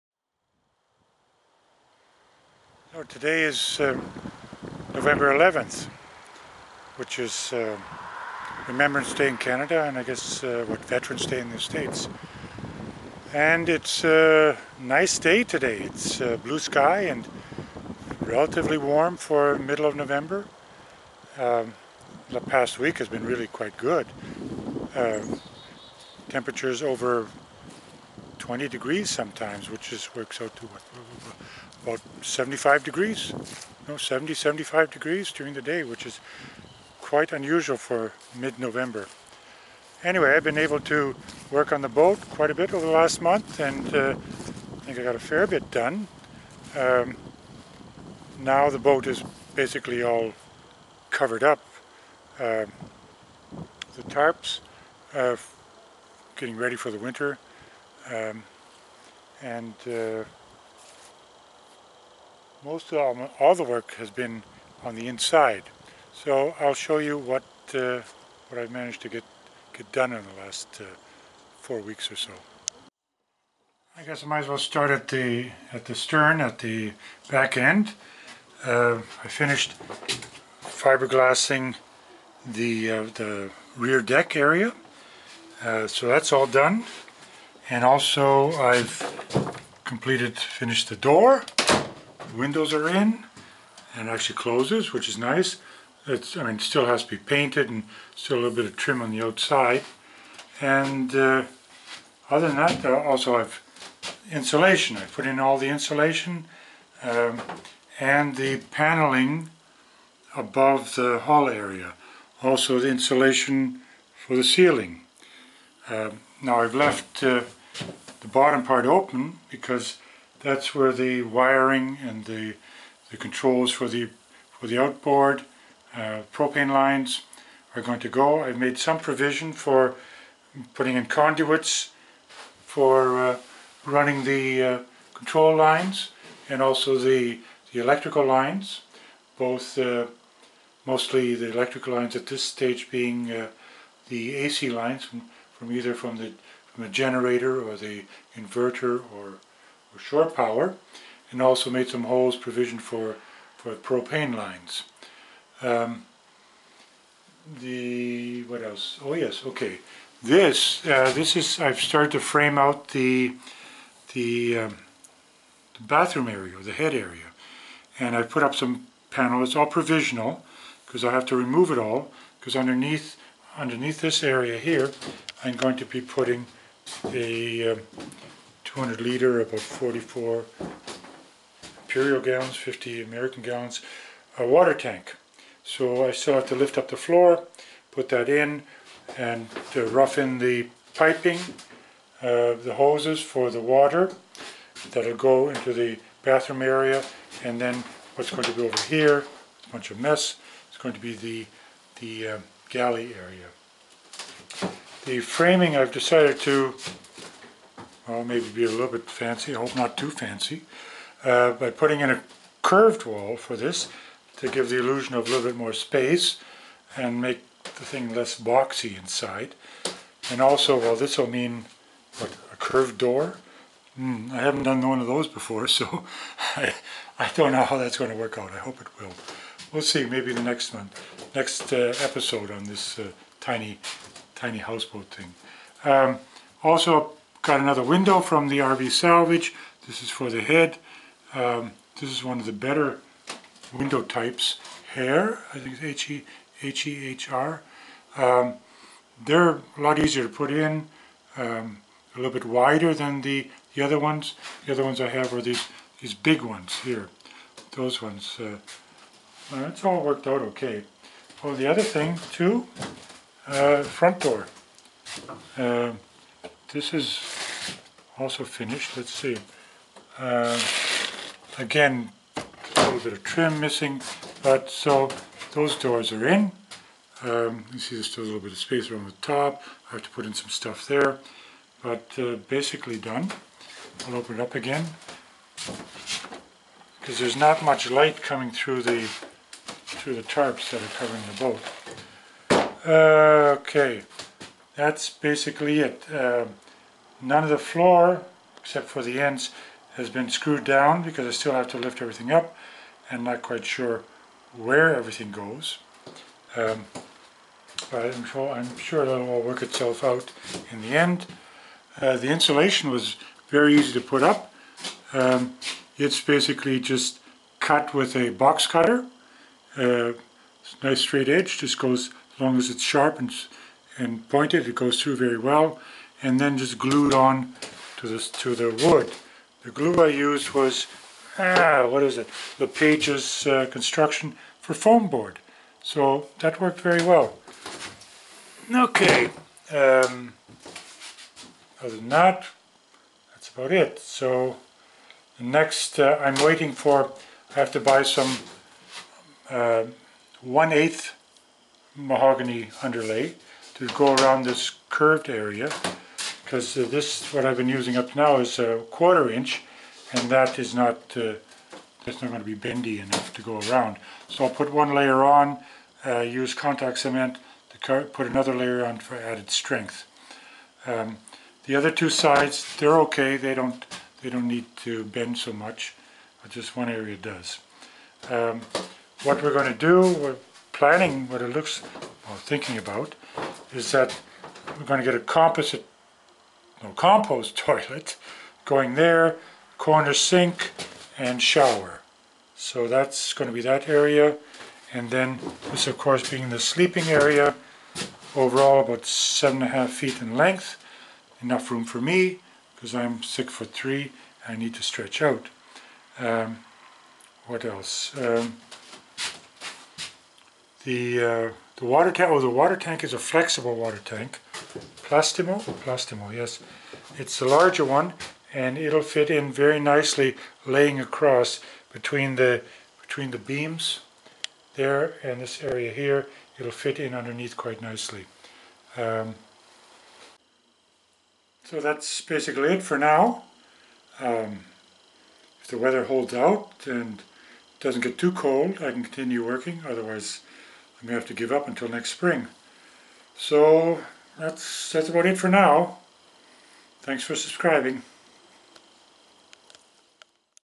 Tiny Houseboat update nov 11
Latest and maybe last update before winter sets in.  Progress since the last update consists of adding insulation along the walls and ceiling, provisionally tacking panels in place above the hull, completed front and rear entry doors and framing the bathroom area.